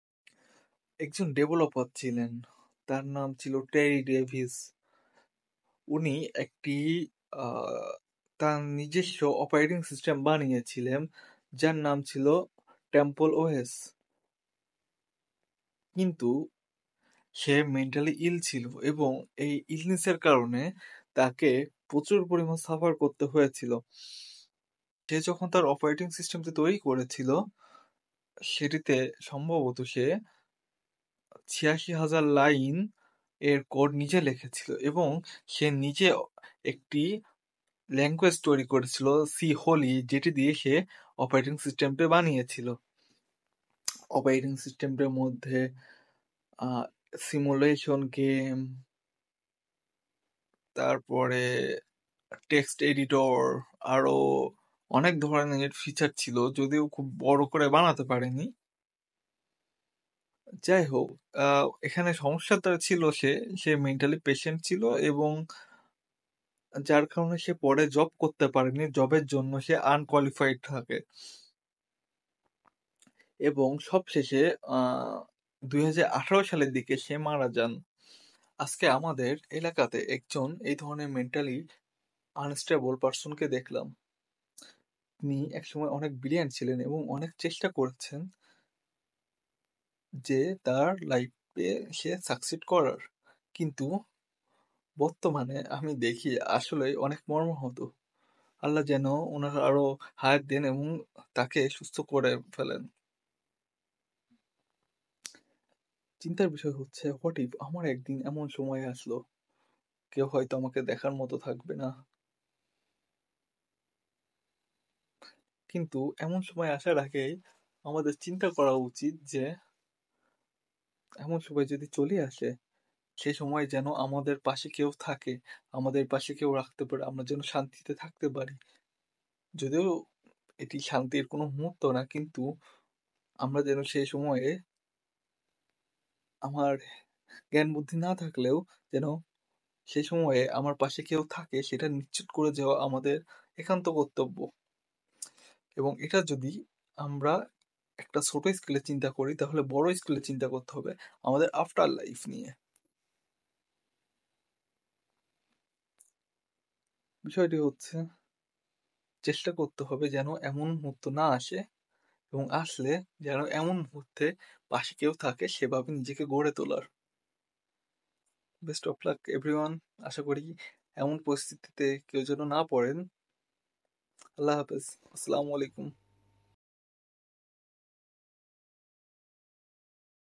Mind Matters: Exploring Mental Health with Terry Davis & Preparing for a Positive Future
Explore the complexities of mental health in this insightful video, drawing inspiration from the life of Terry Davis. Reflect on important lessons, break the silence surrounding mental health, and discover strategies for a resilient future. Join us on this empowering journey to foster understanding and well-being. Your mental health matters, and together, we navigate towards a brighter tomorrow.